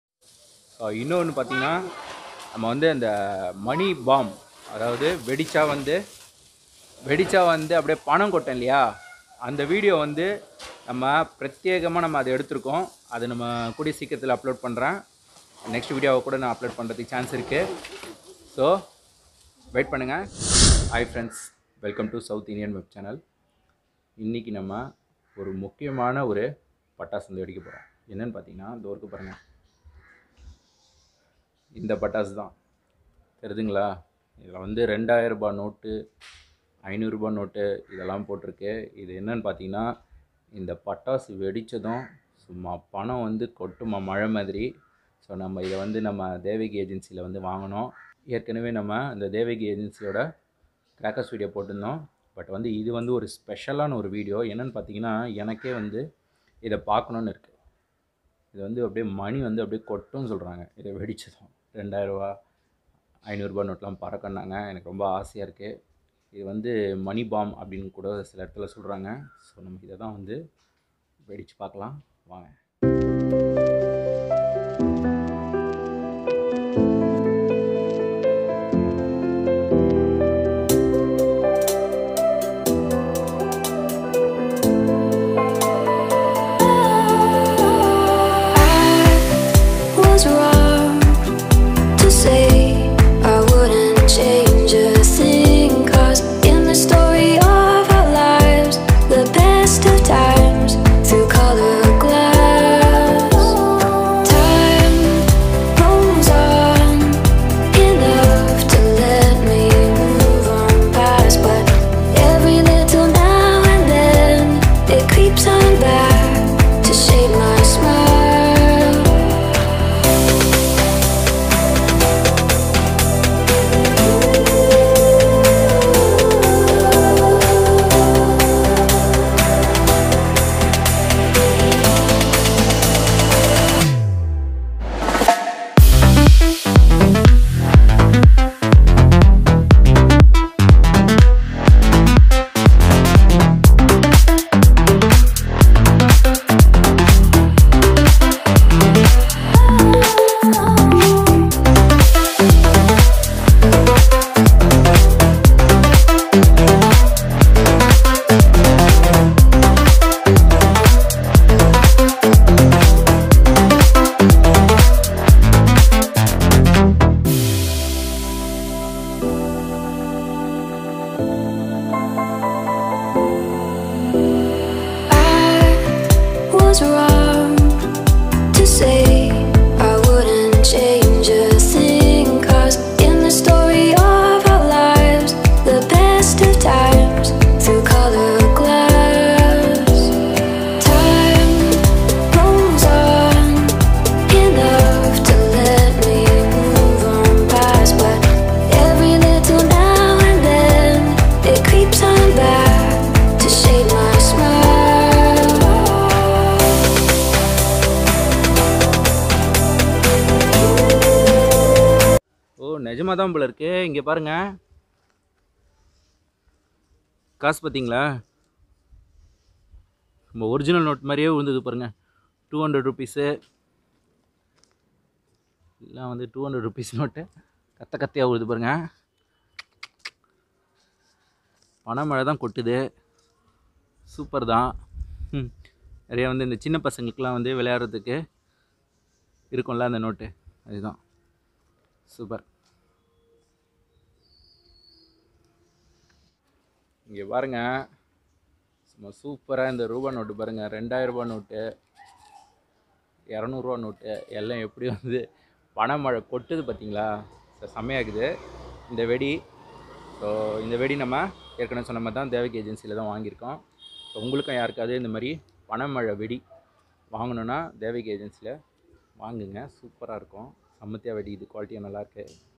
பறக்கும் பணம் பட்டாசு பாண்டிச்சேரியில் | Diwali Money Flying Cracker in Pondicherry
Diwali celebration with Crackers

விளம்பர தொடர்புக்கு
For Advertising & Promotion👇

விளம்பர தொடர்புக்கு
For Advertising & Promotion👇